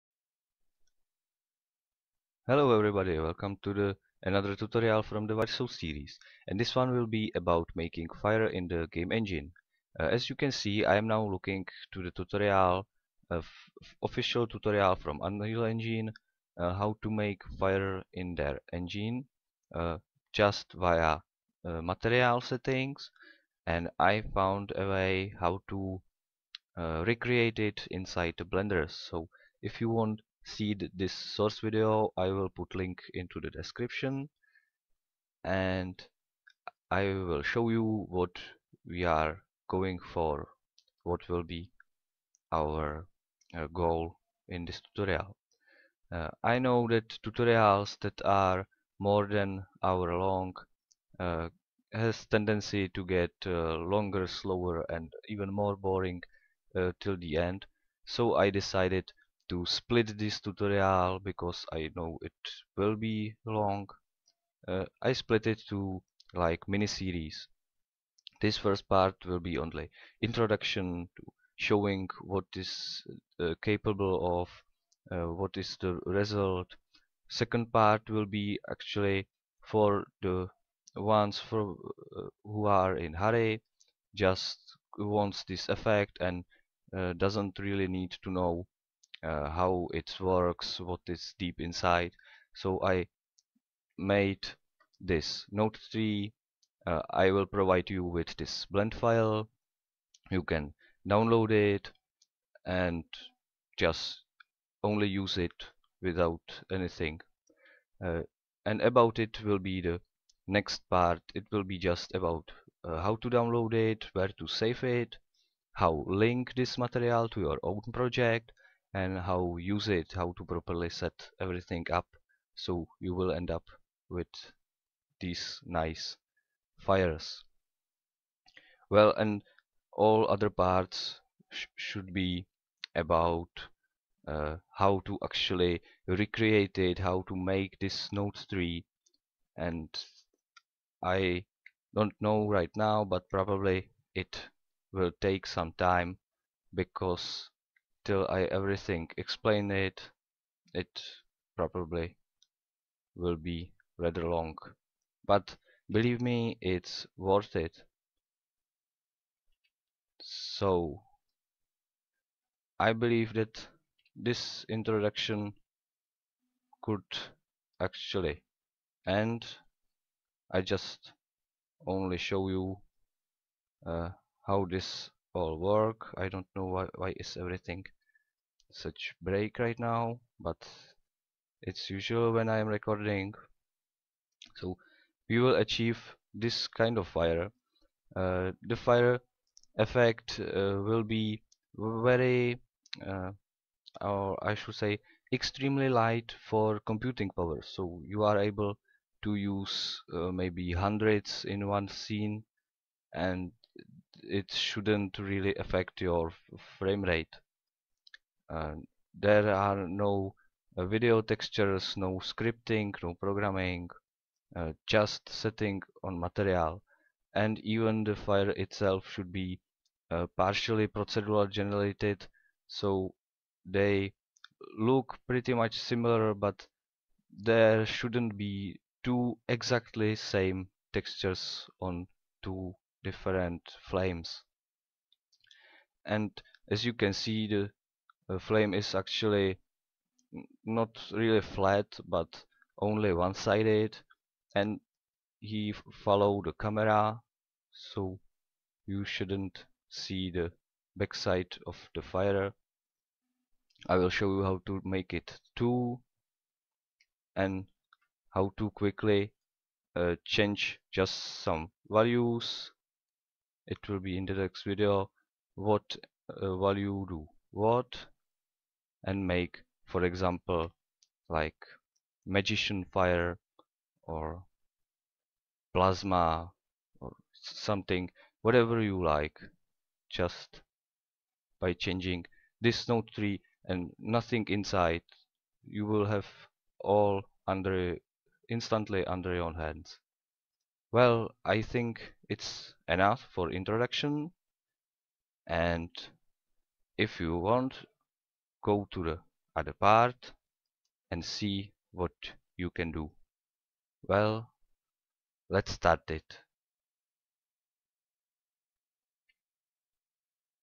WireSoul tutorial - 06/01 - [BGE fire - Introduction]
Introduction to a tutorial miniseries about how to make fire material in Blender game engine. Using just node editor, very light technique for computing power (hi framerate even with lot of flames).